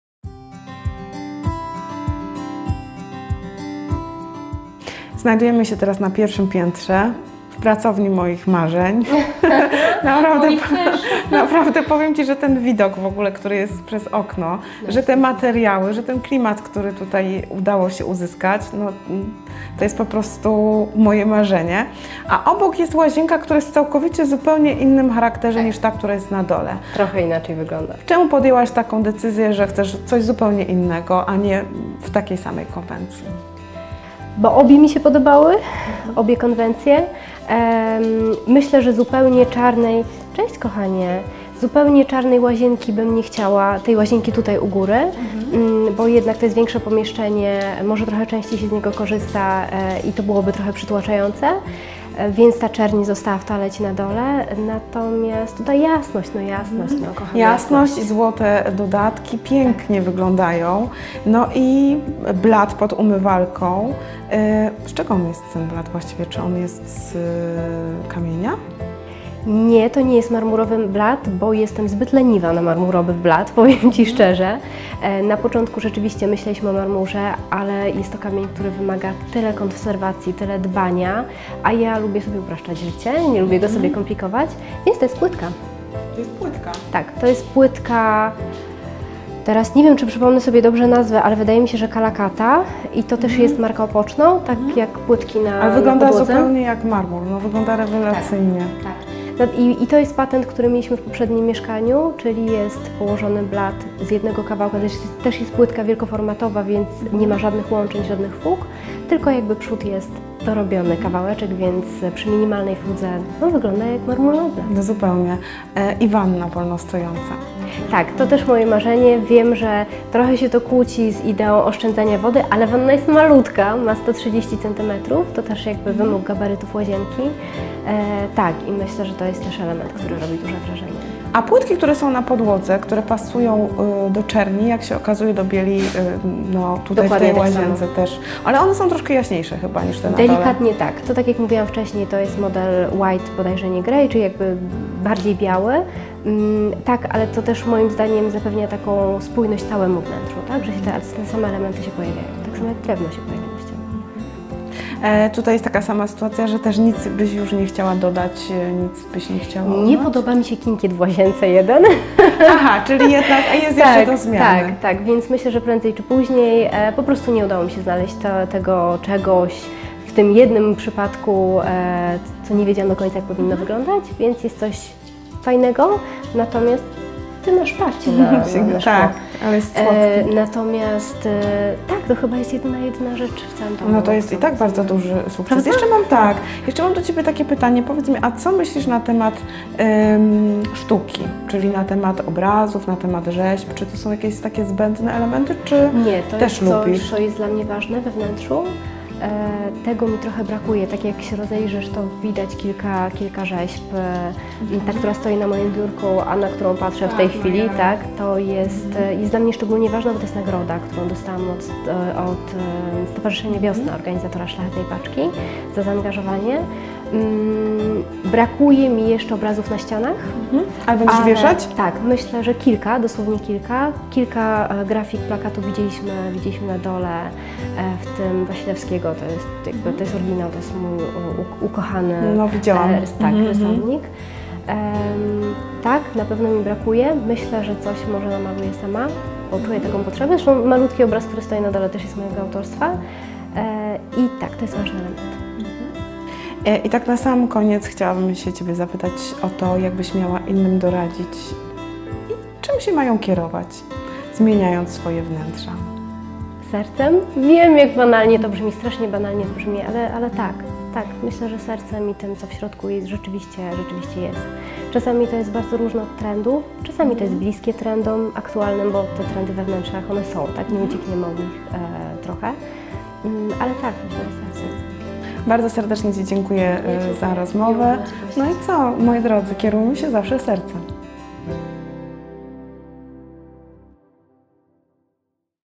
Rozmowy Opoczno-Simplicte cz.3
Pracownia z widokiem, czy marmur jest marmurem i jak pracuje się w domu w lesie. Rozmowa architekta wnętrz Olimpii Ajakaye z Katarzyną Kędzierską, prowadzącą największego polskiego bloga o minimalizmie – Simplicite.pl